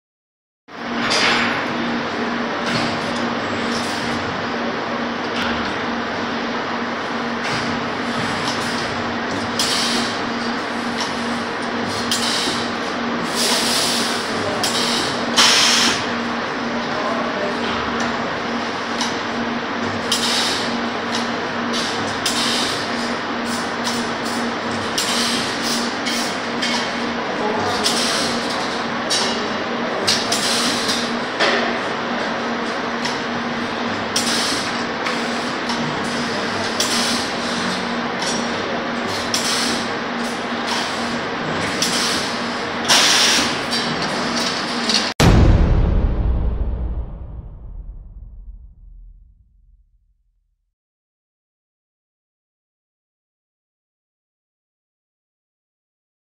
Spot Welding Machine for Steel Wires Welding
The spot welding machine used here shows the technique where the steel wire is welding in a manner to give it a form of a grill. The process here is resistance welding where the two copper alloy electrode is being used. The pressure along with heat at one spot leads on to welding. The process is really an ancient process of resistance welding. The quality of the weld is very good and nugget formation will be there as certain.